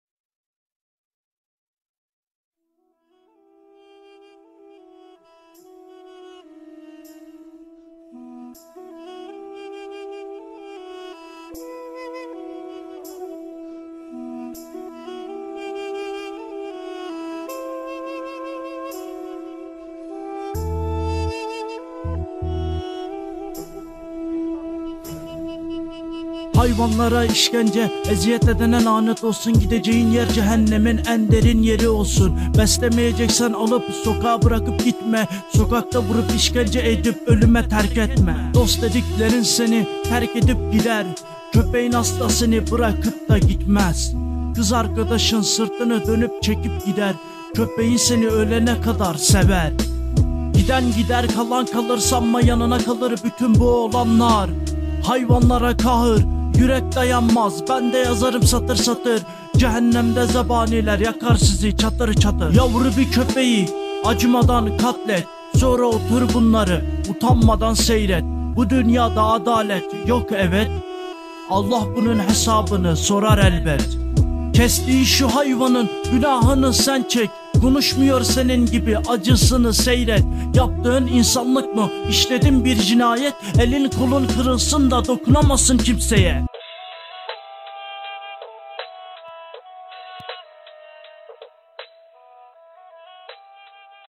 ÖLEN YAVRU ANISINA BATMANDAN RAP GELDİ
BATMANDAN MERT YİĞİT KARDEŞİMİN HAYVAN SEVERLERE HEDİYESİDİR TEŞEKKÜRLER KOCA YÜREKLİ İNSAN
MERT YİĞİT YOU TUBE